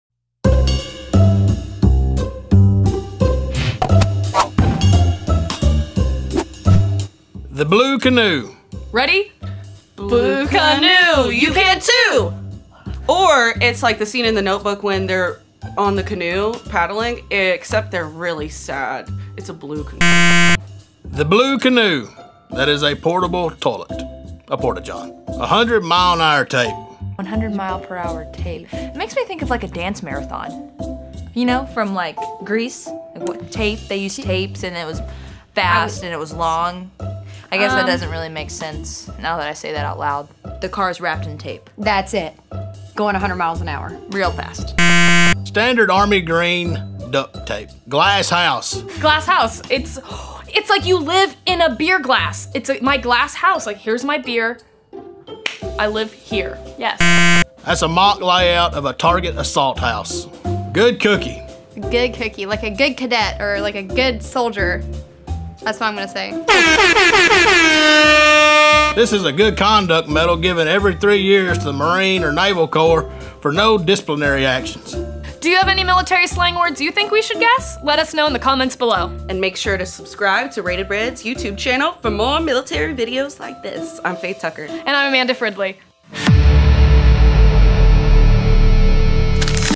Our Hosts Guess Military Slang
Blue Canoe?
100 MPH Tape? Our hosts guess what military slang words mean.
Check this out!!!